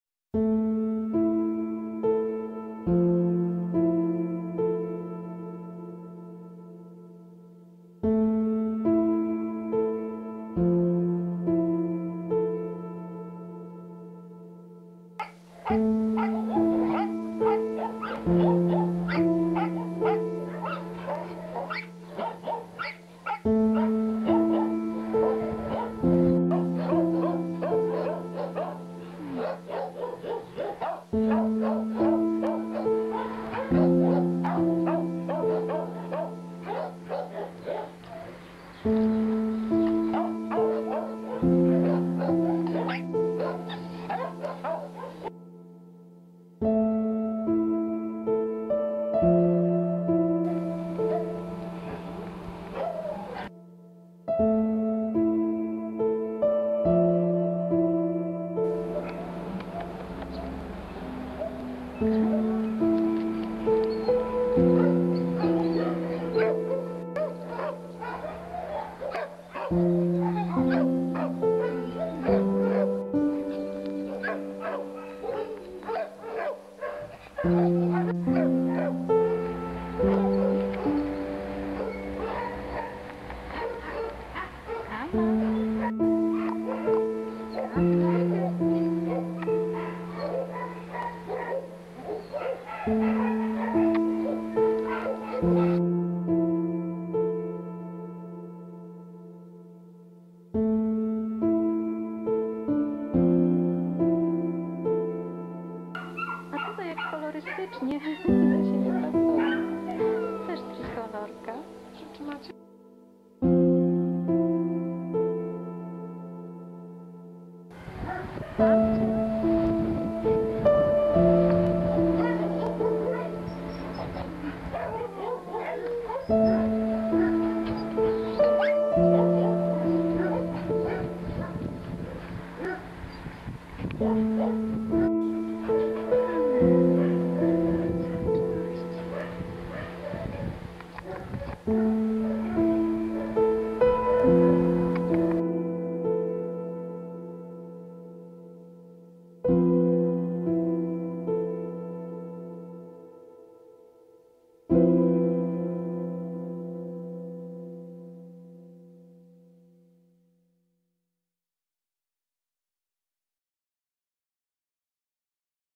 ♥ KAMA ♥ Schronisko dla Bezdomnych Zwierząt w Kaliszu
Kama - bezmiar miłości w zasięgu ręki.
Kama to subtelna psia piękność o aksamitnym futerku i wspaniałym charakterze.
Rozczula spojrzeniem - widać, jak bardzo tęskni za miłością, czułym dotykiem i kochającym człowiekiem. Sunia jest bardzo łagodna.
Kamusia nie wykazuje cienia agresji w stosunku do kotków, może wiec zamieszkać w domu z mruczkami.
Sunia przebywa w schronisku od niedawna (trafiła na Warszawską miesiąc temu).
Jej wiek ocenia się na 3 lata.
Przebywa w boksie C 20, a ostatnie cyfry czipa to: 5410
Jeśli nie możesz adoptować - udostępnij, proszę!
Schronisko dla Bezdomnych Zwierząt w Kaliszu